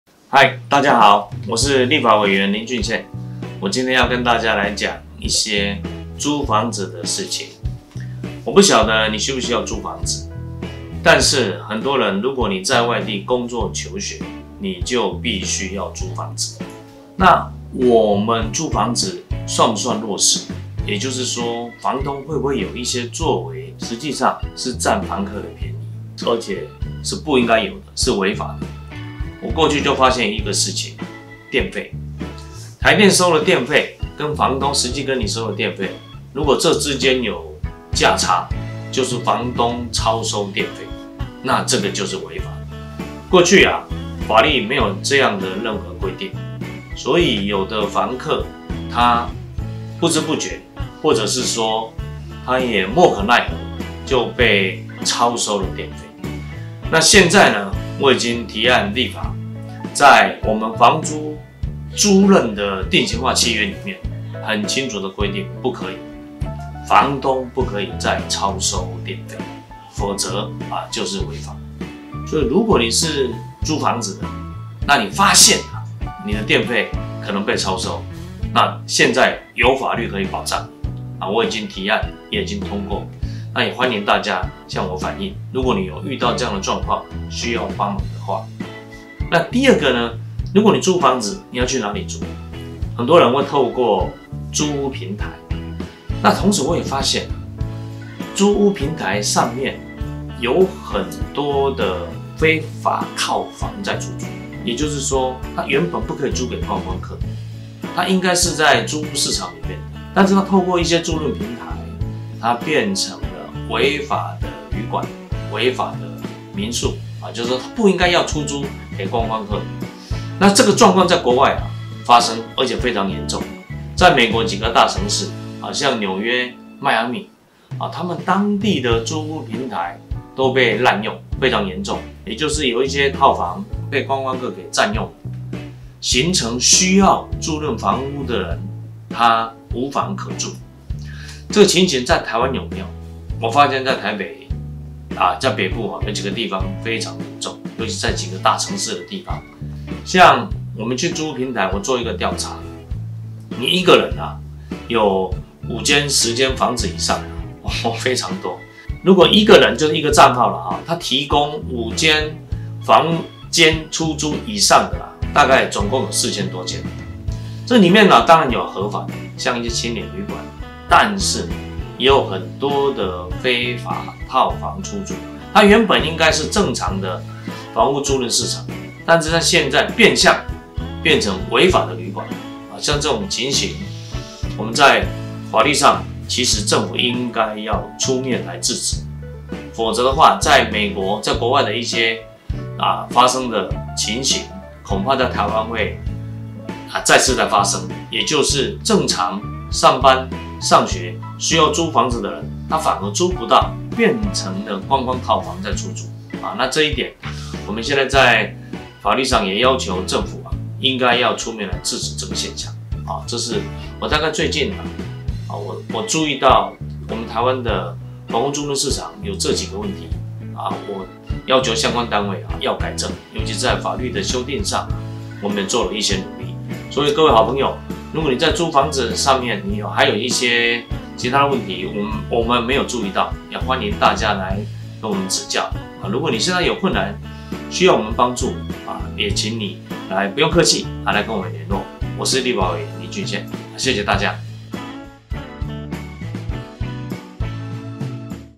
拒絕再被超收電費！俊憲推動的租屋新制終於上路了｜林俊憲
租屋，是每個出外人都會經歷的過程。一個人孤身在外，只求有個遮風避雨之處，因此租屋族面對房東不合理的要求，往往只能接受；其中造成最多人困擾的，便是房東所收的電費高出台電公告一大截，相當不合理。

俊憲看到這個問題，召開記者會呼籲政府訂定出合理規範；現在租屋新制即將上路，只要房東所收電費超出台電公告標準，就是違法！用多少電就付多少錢，合情合理。

除了微觀的電費問題外，俊憲也發現近年來網路短租平台的興起，使得原本應該進入租屋市場的房源變相成為違法旅館，壓迫到租屋族的權益；俊憲也已要求政府單位嚴查這類情形，避免出現「有空房卻租不到」的窘境。

出外打拚，有俊憲做你的後盾！如果在外租屋還有遇到任何問題，都歡迎向俊憲反映。